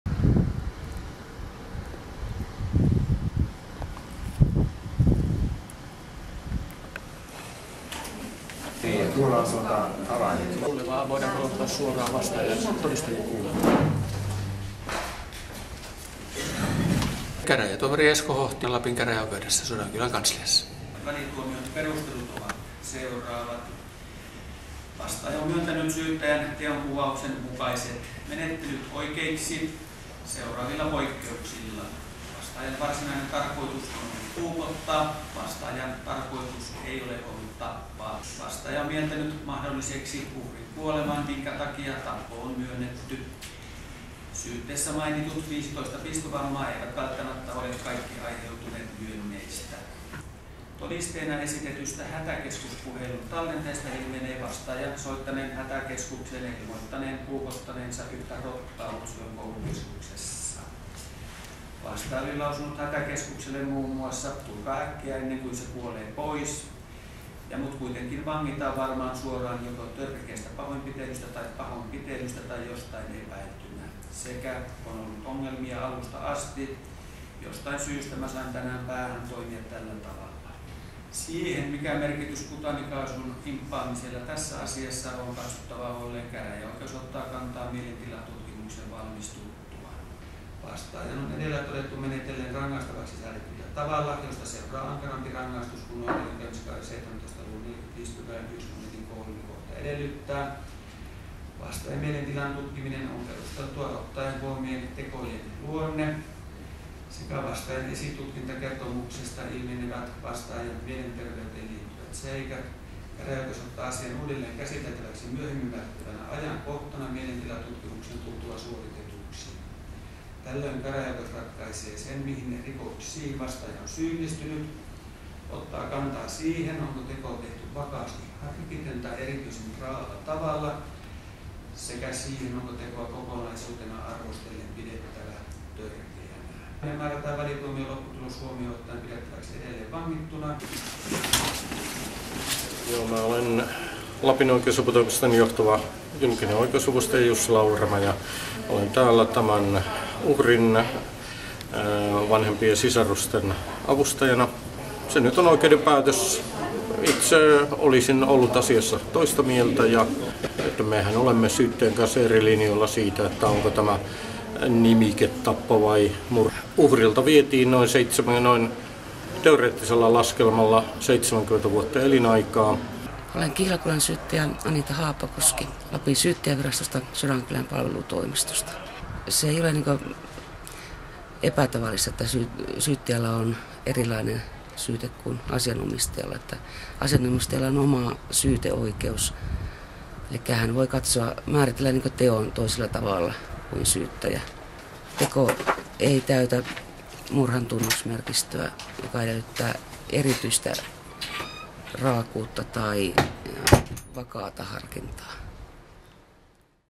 Utsjoen oikeus
Utsjoella 24. lokakuuta 2014 tehdystä koulusurmasta syytetty 31-vuotias mies on määrätty mielentilatutkimukseen. Lapin käräjäoikeuden antaman välituomion mukaan mies on syyllistynyt henkirikokseen, jossa kuoli 16-vuotias saamelaislukion oppilas. Video: Olli Miettunen / Lapin Kansa.